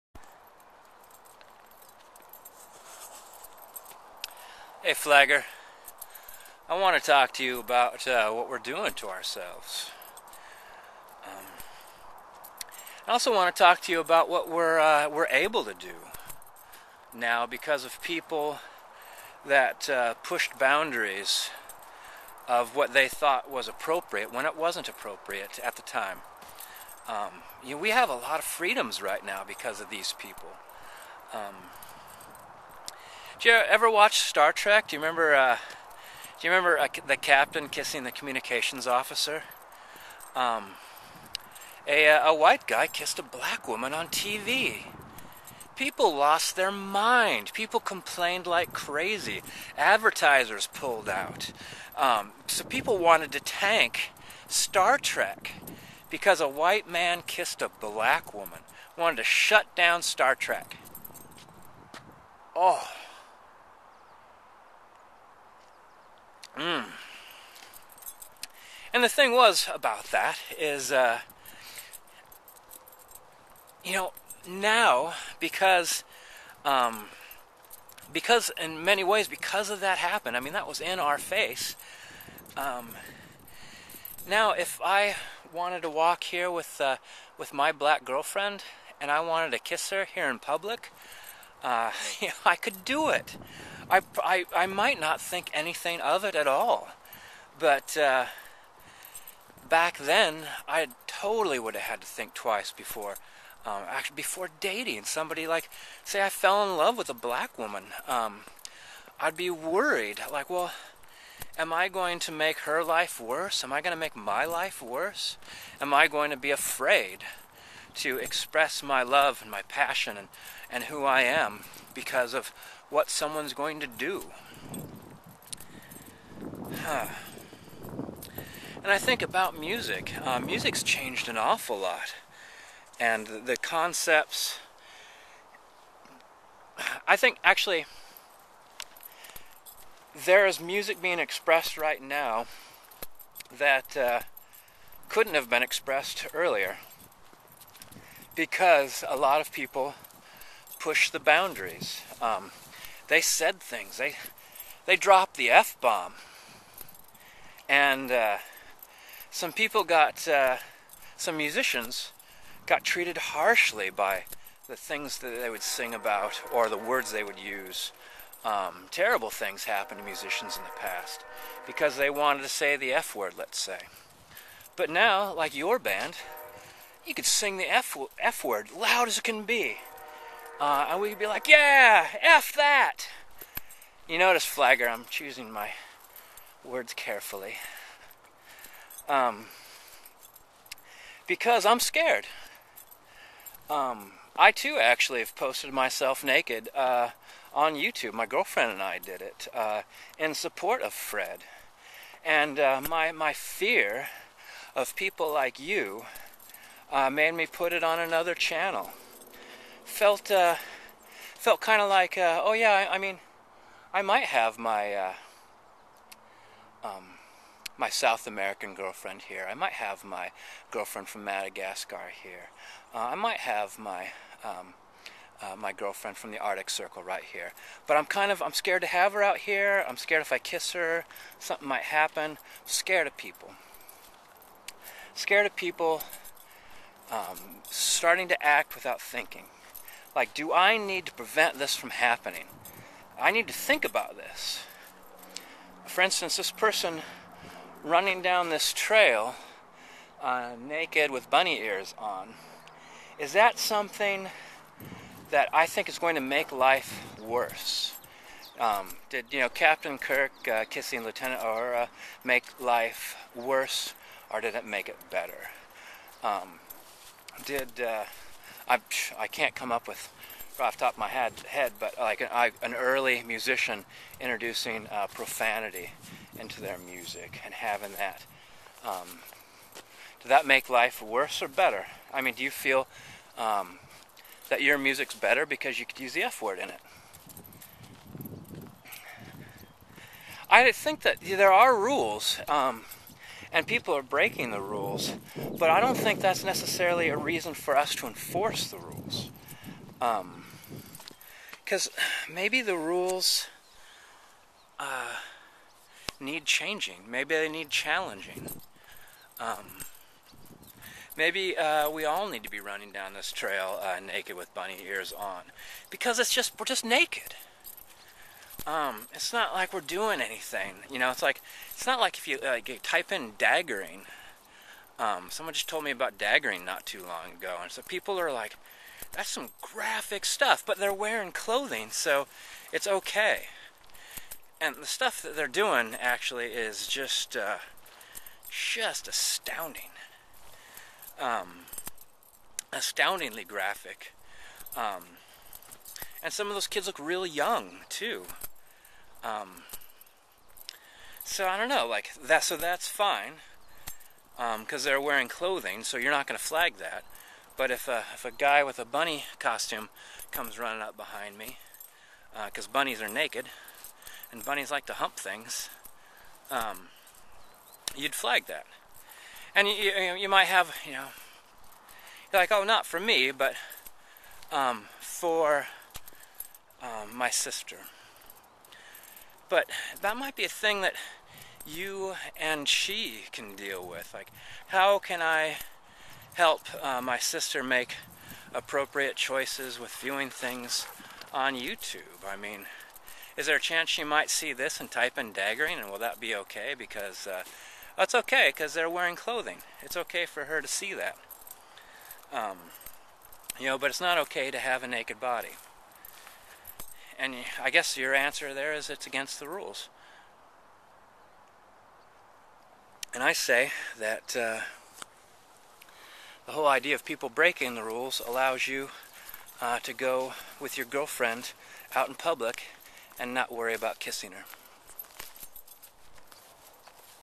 Dear Flagger
just think my friend. who's side are you on, that of the creative or that of the destructive. you live a life paved by the suffering of artists and free thinkers, and you are now going to try to keep them down? think, then act. express yourself in any way you can, as long as you dont hurt anyone or keep them from expressing themselves. how about that for a "guild line"?